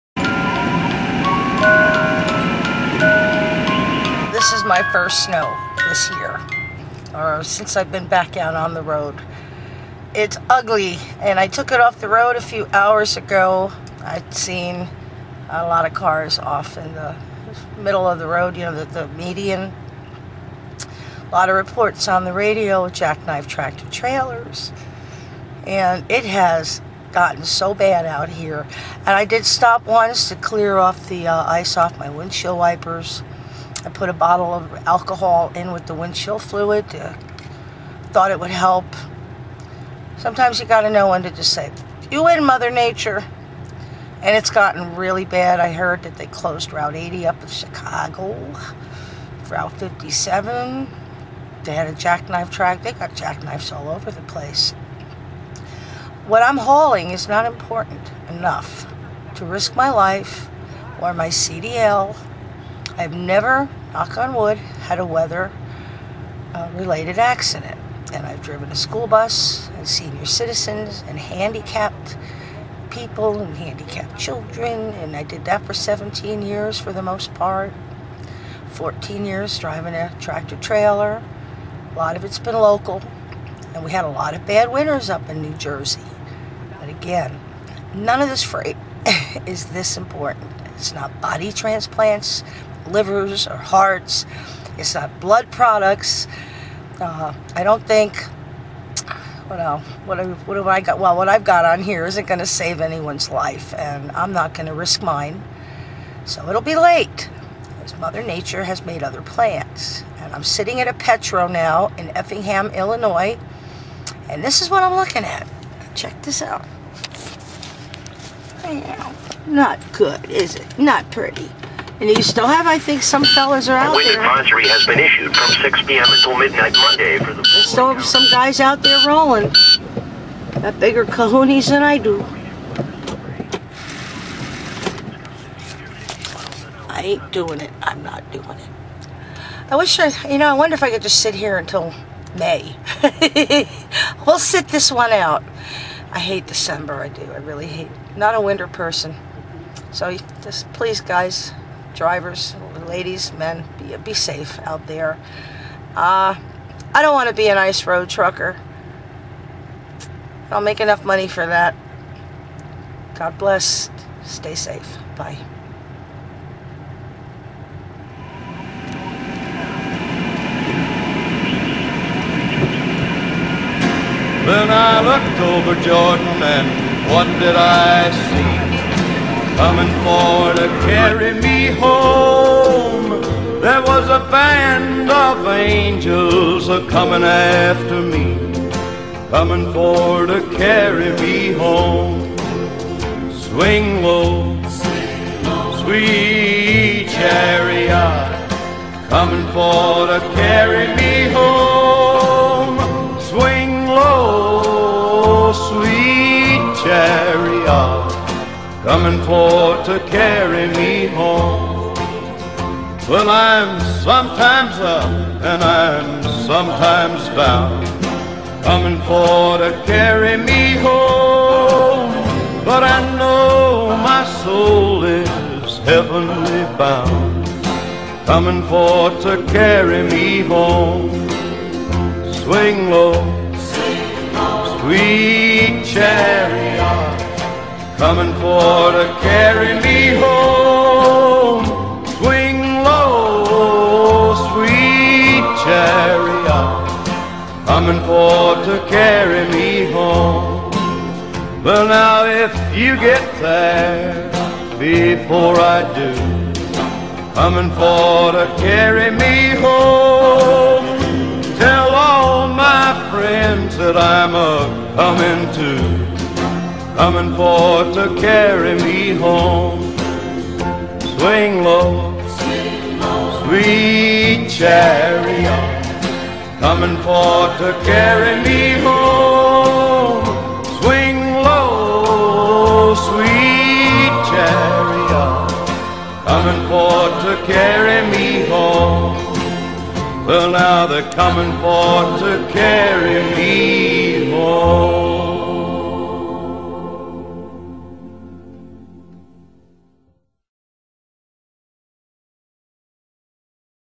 Truck wrecks
Illinois got battered by a bad snowstorm on 12/12/10.  I took my big rig off the road early, before noon.  Only one hour later, reports crackled over my CB radio...
"Jack-knifed tractor trailer at the 160", only 1 mile away from where I shut down.
The next day, Interstate 70/57 looked like a combat zone!  I heard that one driver died when a tree limb came smashing in through the driver''s side window.
   Pictures of the aftermath are included in this video.  Snow, drive slow!
Ice, NO DICE!!  Know when to take it off the road, drivers!  Don't risk your CDL OR your life!!!